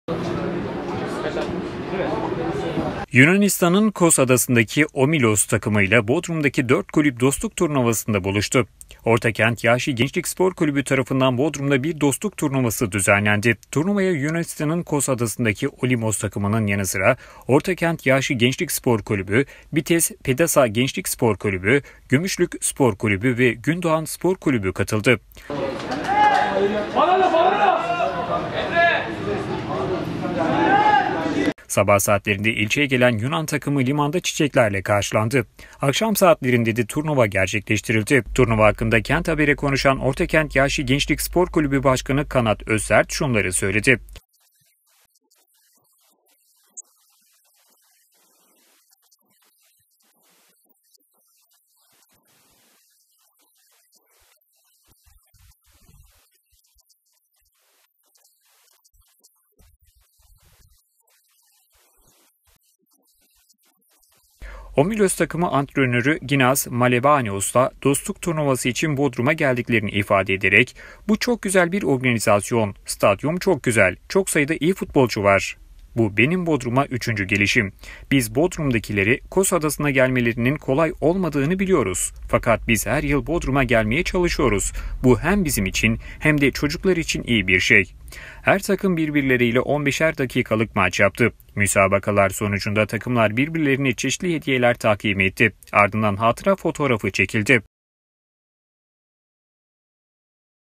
BODRUM – KOS DOSTLUK TURNUVASI YAPILDI
Yunanistan’ın Kos (İstanköy) Adası’ndaki Omilos takımı ile Bodrum’daki 4 kulüp, dostluk turnuvasında buluştu.